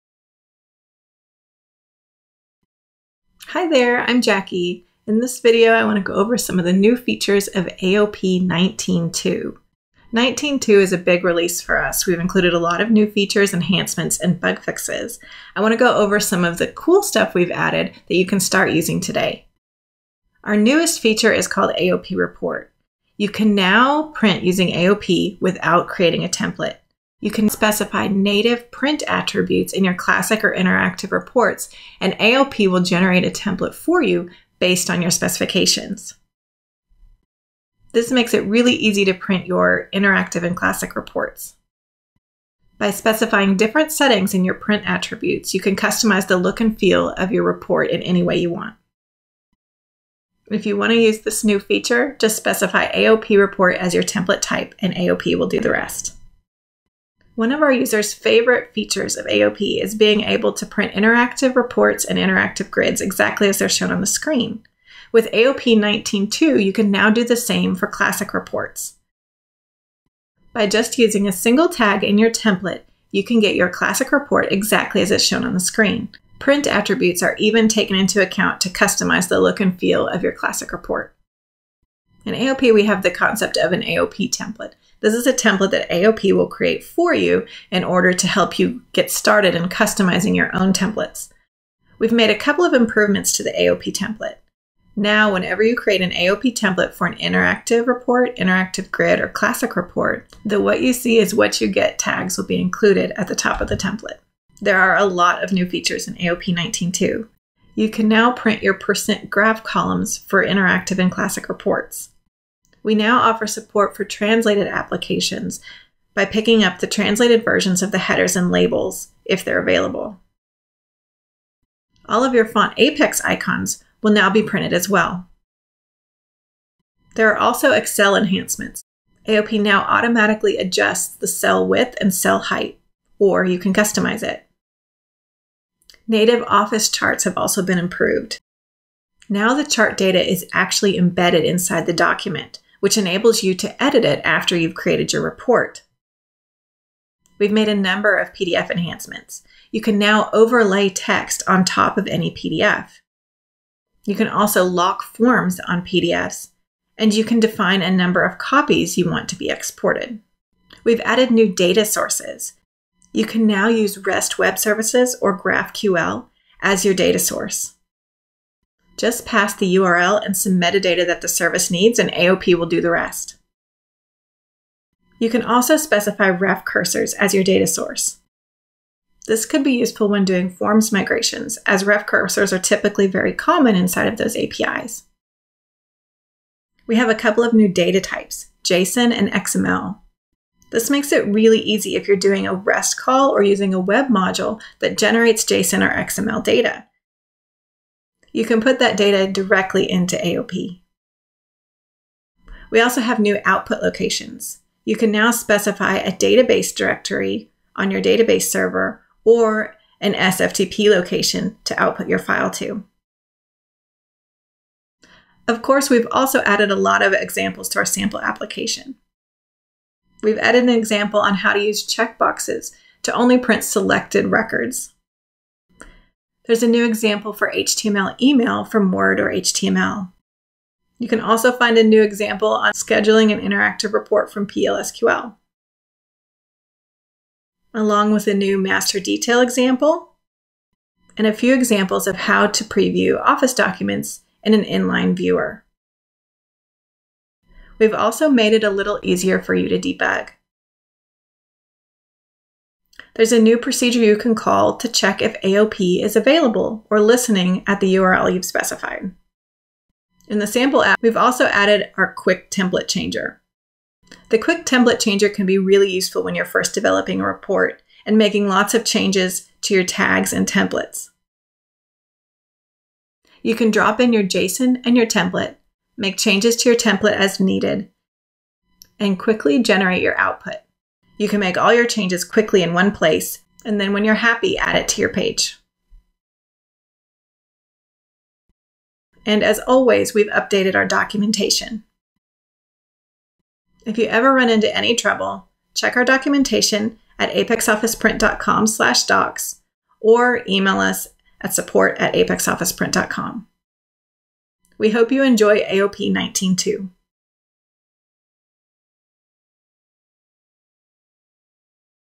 APEX Office Print (AOP) 19.2 - New Features
This video is a quick overview of some of the new features and enhancements included in AOP version 19.2 (released July 2019).